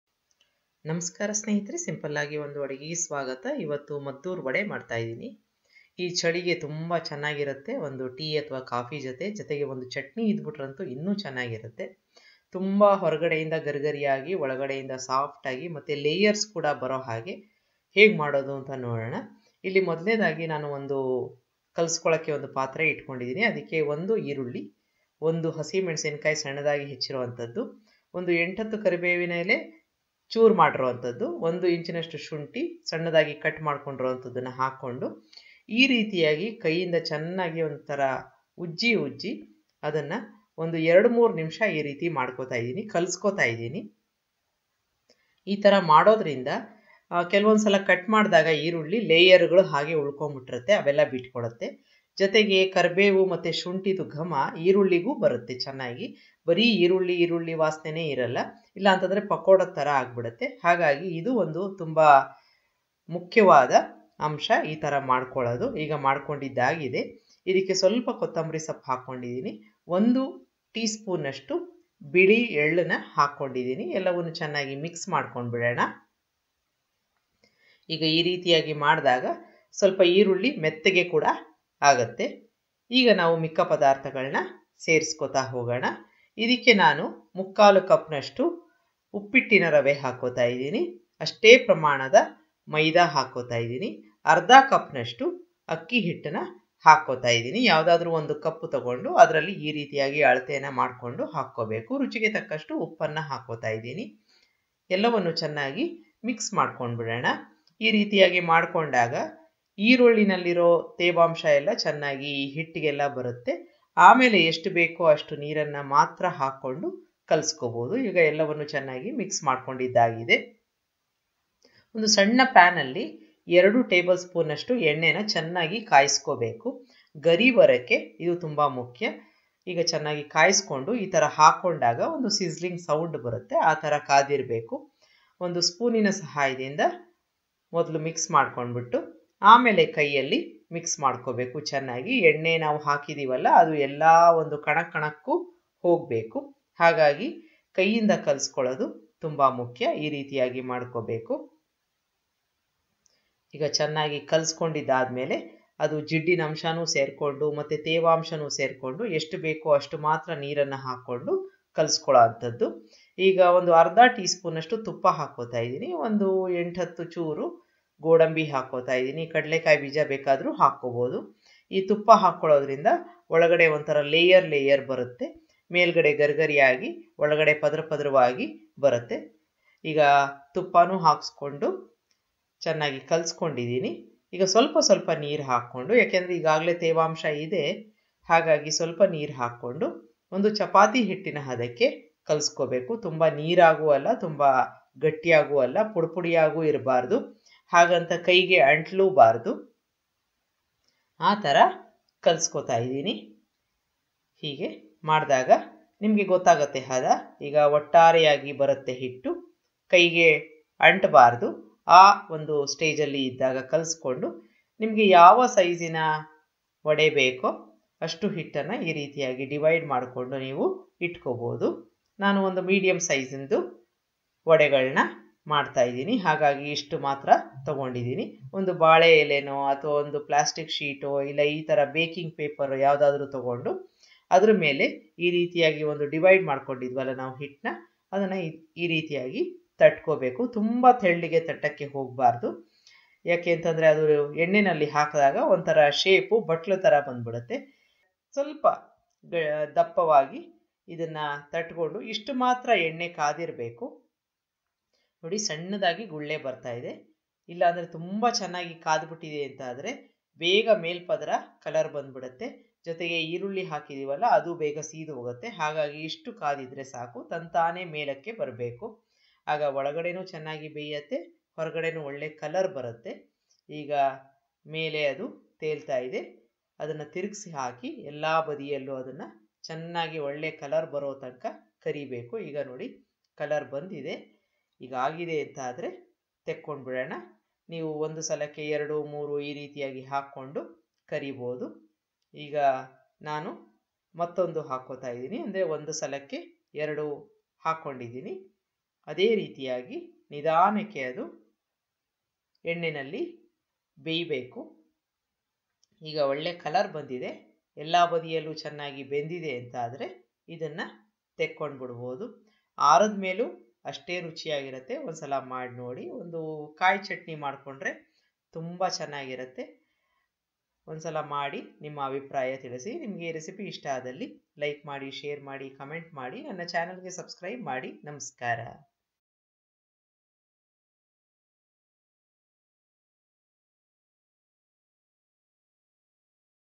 ಮದ್ದೂರ್ ವಡೆ |ಚುಮು ಚುಮು ಚಳಿಗೆ ಬಿಸಿ ಬಿಸಿ ಮದ್ದೂರ್ ವಡೆ | ಗರಿ ಗರಿ ಮದ್ದೂರ್ ವಡೆ | crispy maddur vada
ingredients:
1 onion chopped
1 inch ginger chopped
1 green chilli chopped [ more or less ]
1 tsp sesame seeds
1/2 tsp ghee
2 tbsp oil [ well heated ]
salt
3/4 cup sooji [ upma ]
3/4 cup maida
1/2 cup rice flour
coriander little
curry leaves chopped
few cashew nuts  or peanuts [ roasted ]

* take a bowl add onion , curry leaves and ginger mix  till blends well
* add chilli , sesame seeds coriander  mix well
* add soji , maida and rice flour , salt too
* mix everything and add hot oil combine well
* add ghee , cashew nuts mix well
* add little water at a time knead  soft dough
* divide it into small balls and take baking paper make small disk out of it
* once oil is moderately hot slip disks into oil and fry till crisp and cooked from within
* once golden brown , take out and serve with chutney